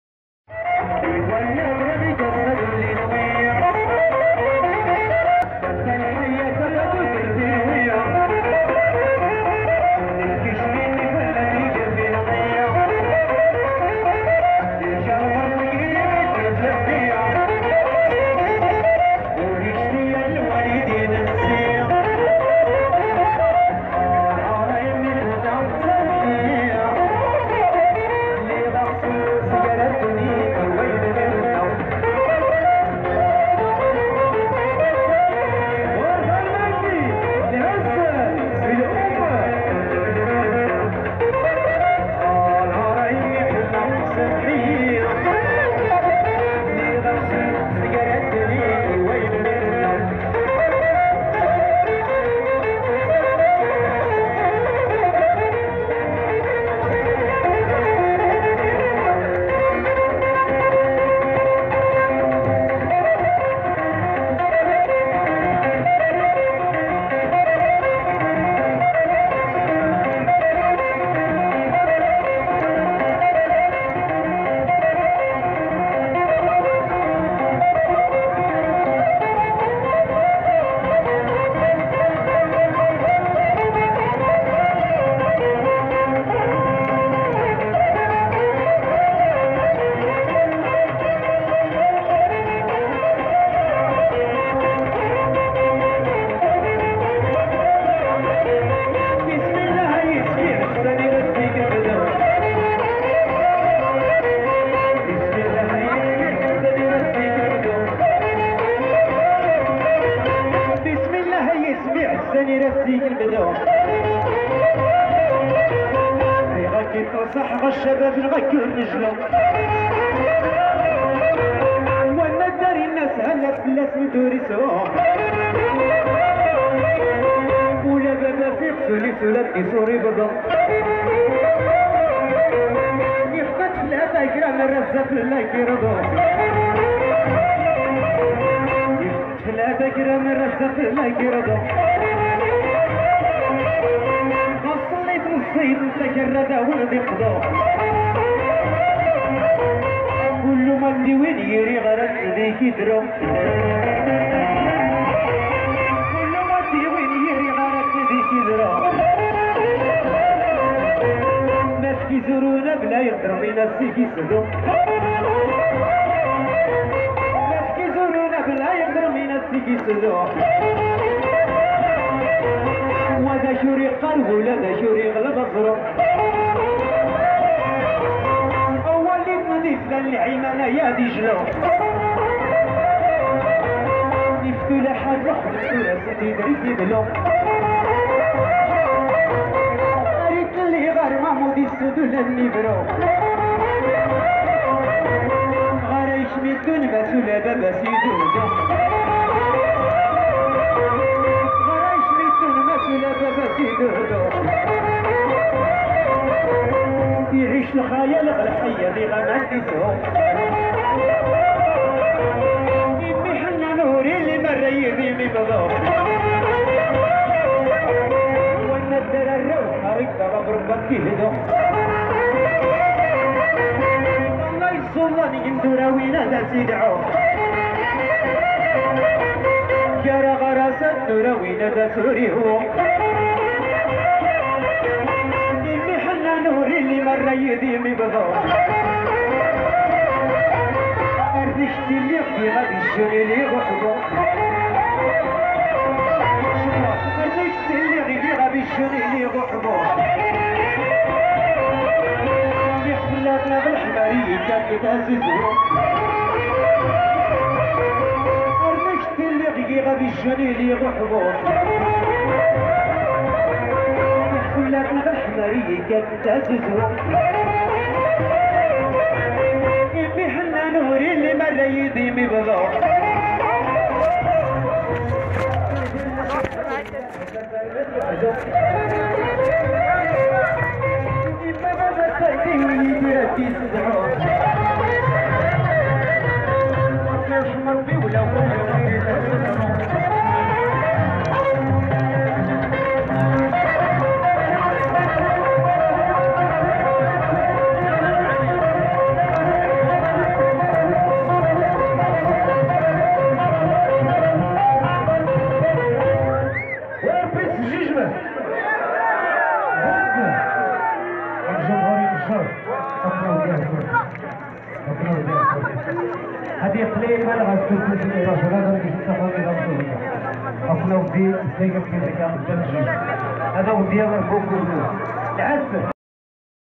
مهرجان اركان الثقافي والفني بتغيرت 2018
تغطية مباشرة من قلب منطقة تغيرت التابعة الى عمالة سيدي افني